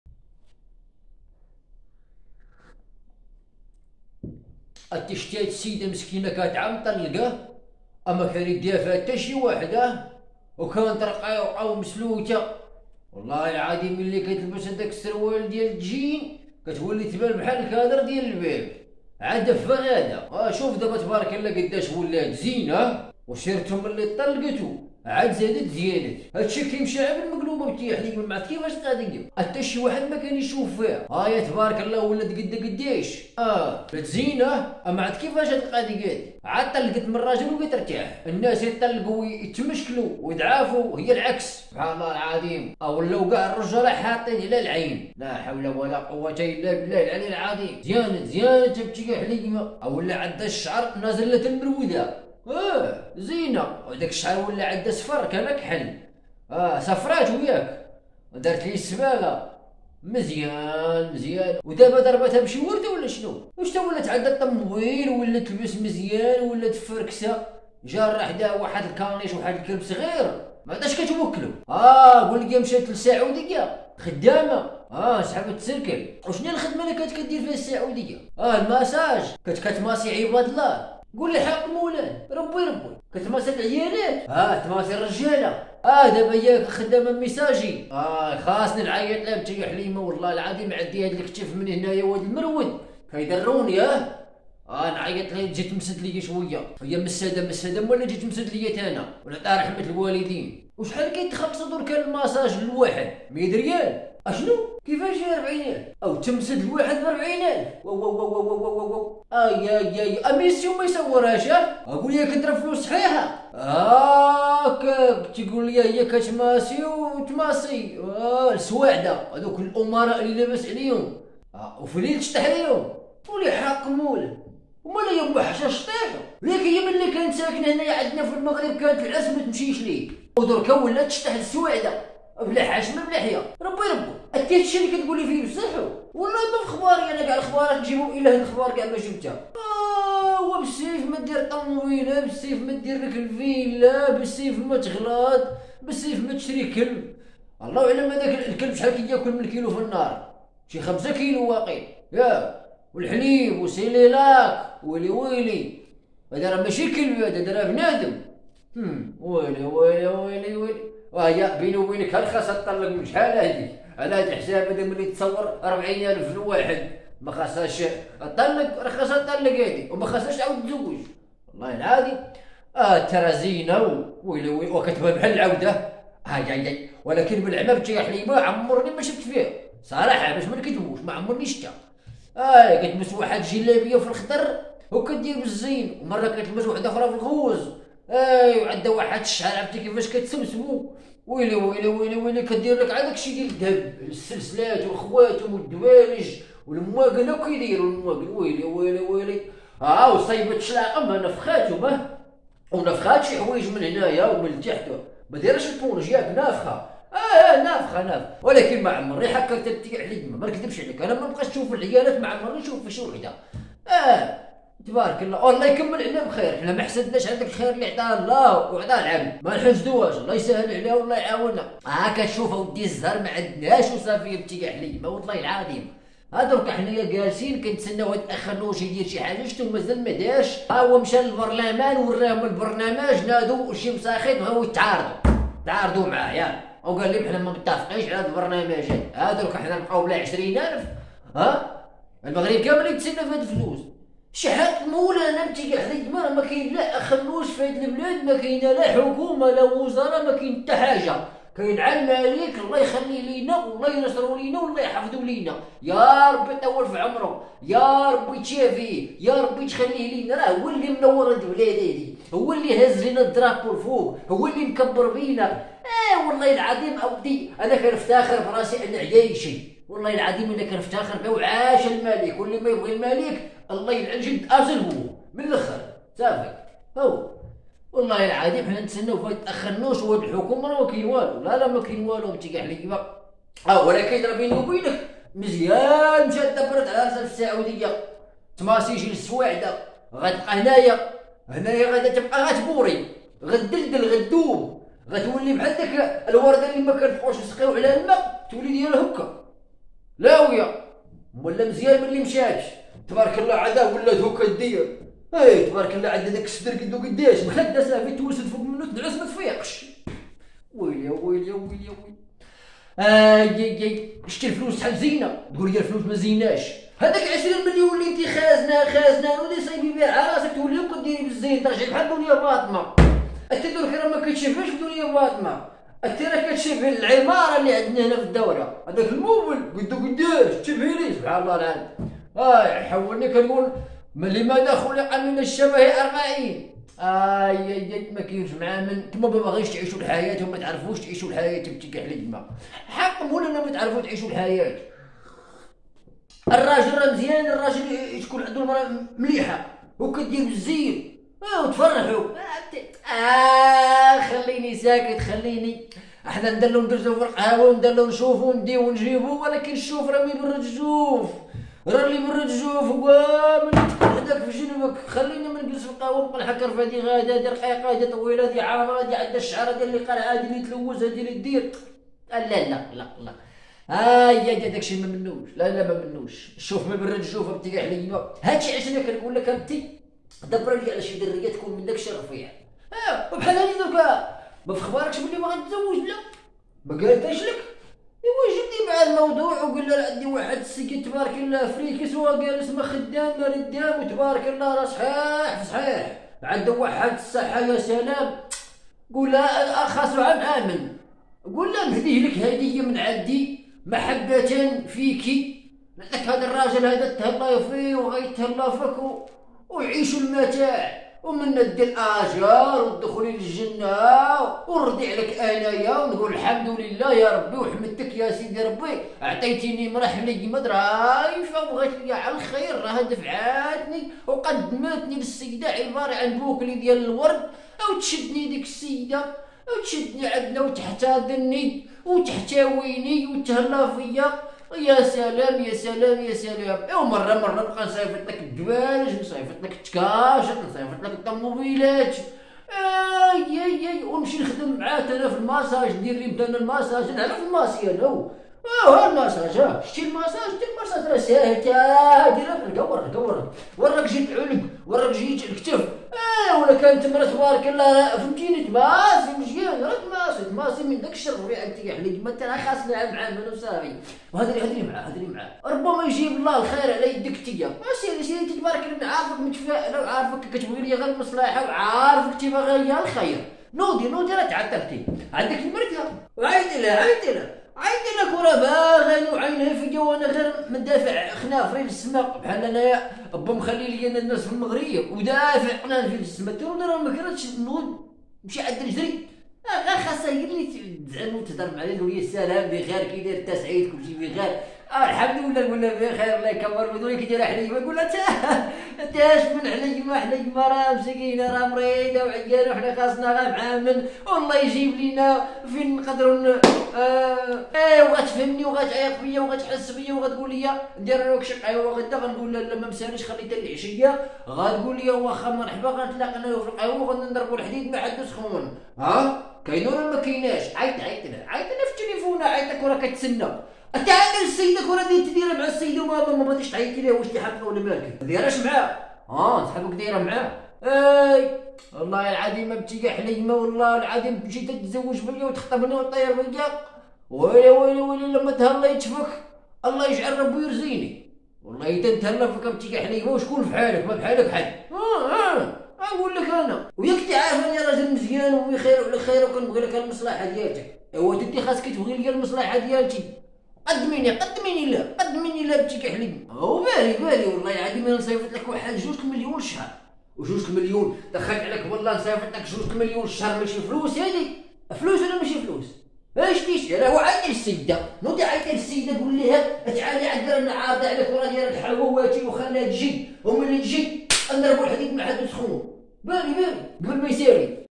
المغربيات في الخليج حصلوهم كيديرو ... داك الشي عاين باين | فكاهة مغربية
المغربيات في الخليج حصلوهم كيديرو ... داك الشي عاين باين | هو عنوان هدا  الفيديو الدي يناقش موضوع مهم جدا حول العديد من الفتيات المغربيات يعيشن في الدول الخليج يعملن من أجل لمقة العيش . ويأتي هد المحتوى في اطار فكاهي وكوميدي للمشادين الكرام.